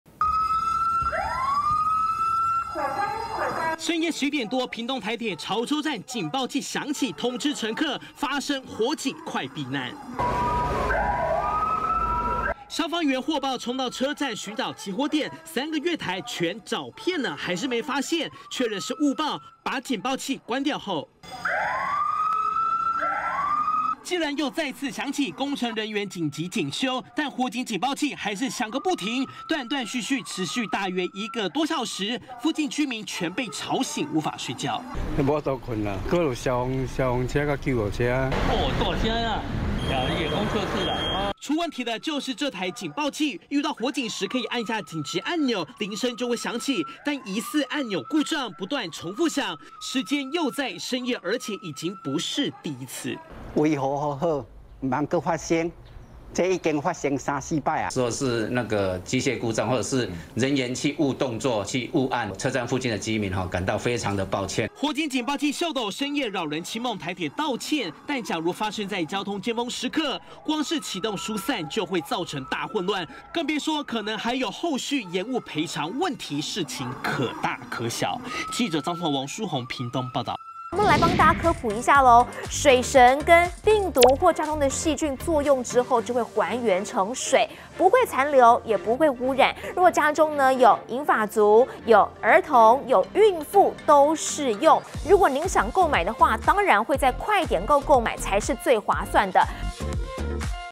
【每日必看】疑故障?吵到睡不著 潮州車站深夜"火警狂響"@中天新聞CtiNews @毛球烏托邦MaoUtopia 20220418
台鐵潮州站，深夜11點多，火警警報器大響，消防員火速衝到車站，找了老半天，都沒找到起火點認為是誤報，但警報器又斷斷續續響了1個小時，附近居民被吵到睡不著，抱怨說類似情況已經3.4次了，真的很擾民。